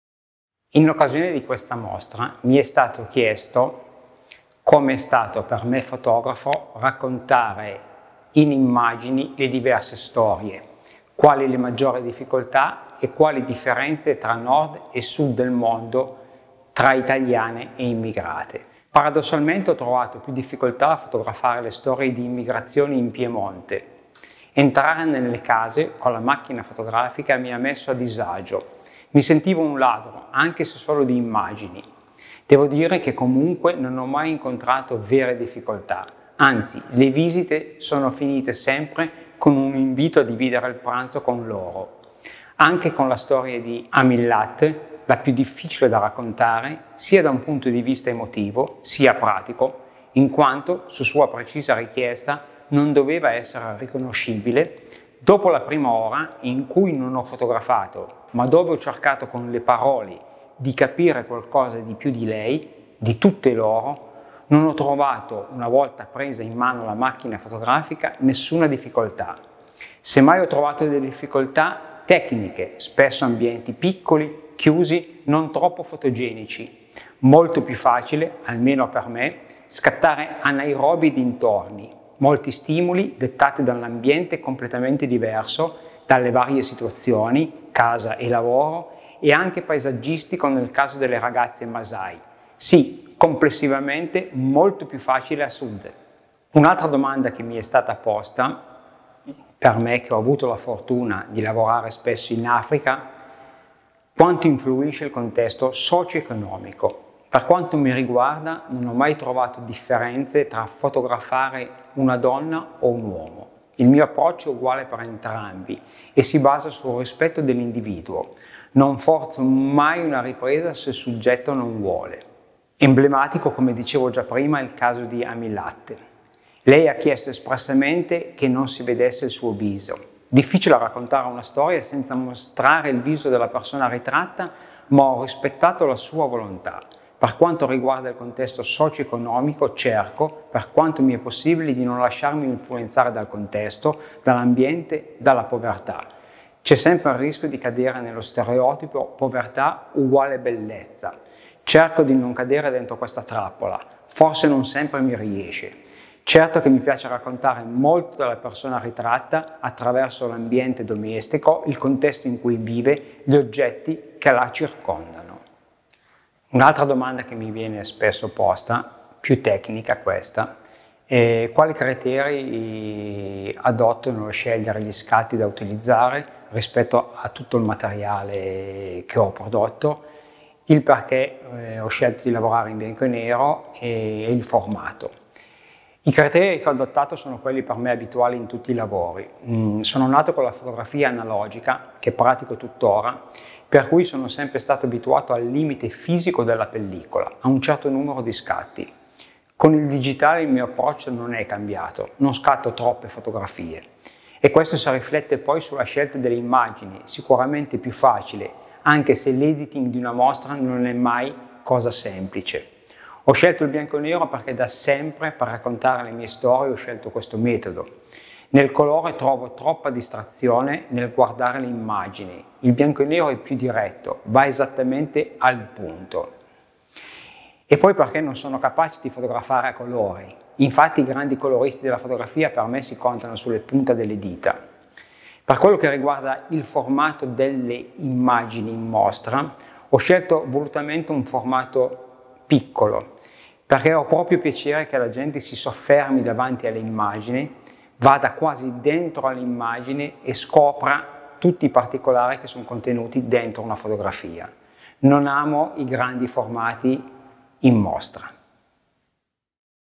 Intervista ad Enrico Minasso - Fotografo
Enrico Minasso ci racconta come ha affrontato il reportage fotografico che ha portato alla realizzazione della mostra "Il femminile di uguale".
Riprese e montaggio: Andrea Chiesa è Progetto Immagine.
Una produzione: World Friends - Crescereinsieme